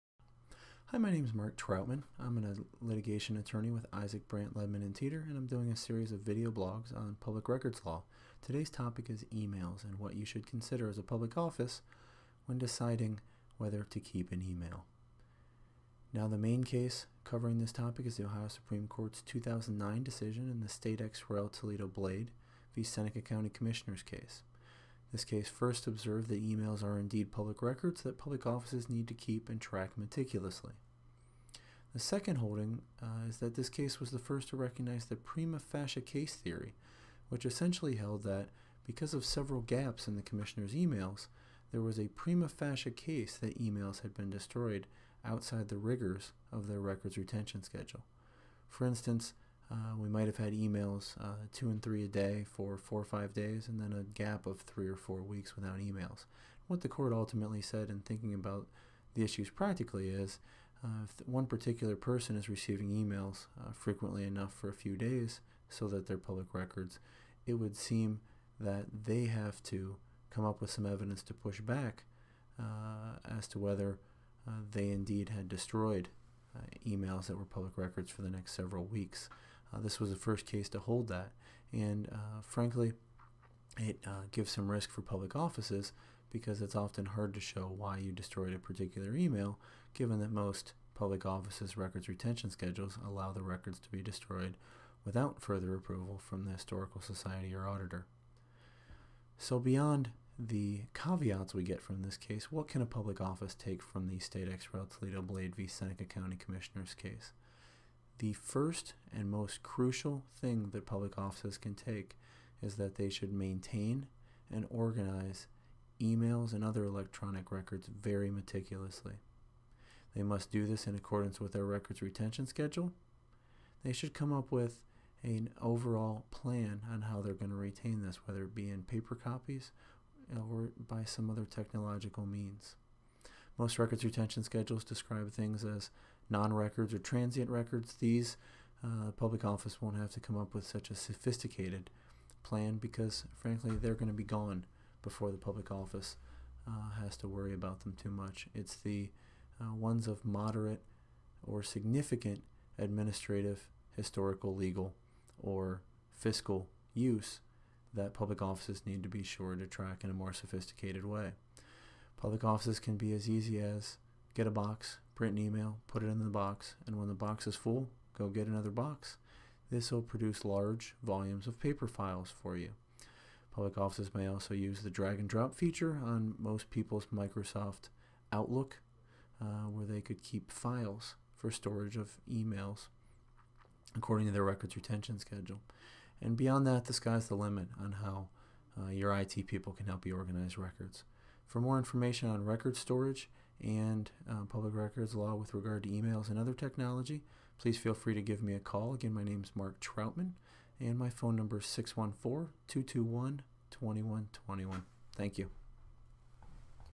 Emails are public records
Should public offices worry about emails when dealing with public records?  The easy answer is yes, but it is much harder to know how to handle the thousands of emails that each of your employees receive every year.  Attorney Mark H. Troutman offers some basic suggestions on how to attack this issue.